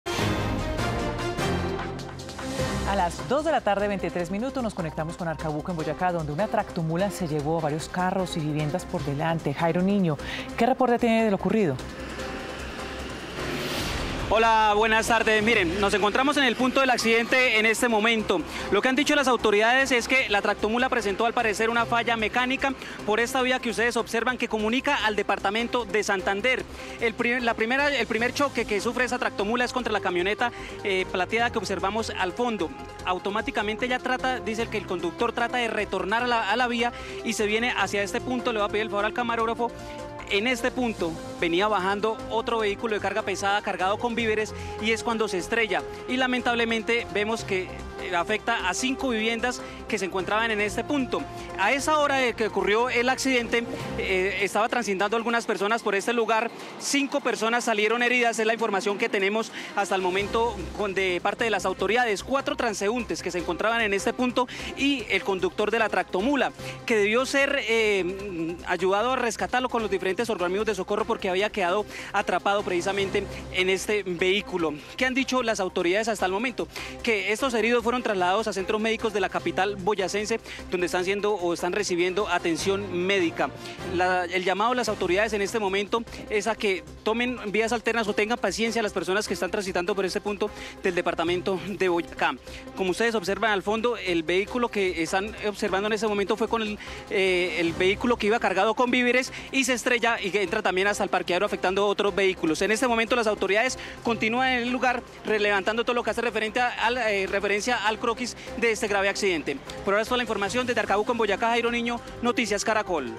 Accidente en Arcabuco: tractomula se fue contra carros y casas
Un grave accidente en Arcabuco, Boyacá, se presentó este jueves 15 de septiembre por una tractomula cargada de carbón que terminó volcada en la vía que conecta a Tunja con Bucaramanga.

Información preliminar indica que el vehículo de carga perdió el control y se llevó todo a su paso. Se habla de unos seis carros y tres viviendas afectadas por el siniestro.

Una falla mecánica, posiblemente de frenos, es la principal hipótesis del siniestro.